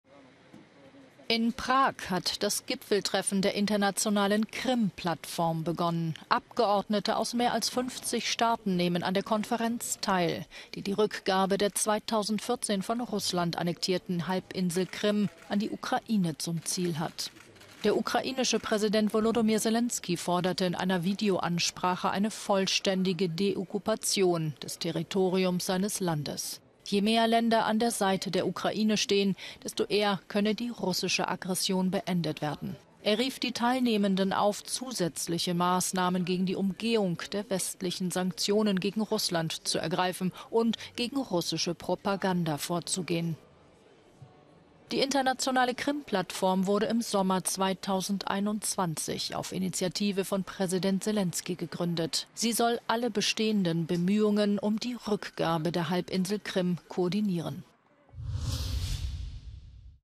Selenskyj: "Die russische Flotte flieht schrittweise von der Krim"
Selenskyj hob auf die erfolgreichen ukrainischen Drohnen- und Raketenangriffe auf russische Stützpunkte auf der Krim hervor. Die russische Flotte würde "schrittweise von der Krim fliehen".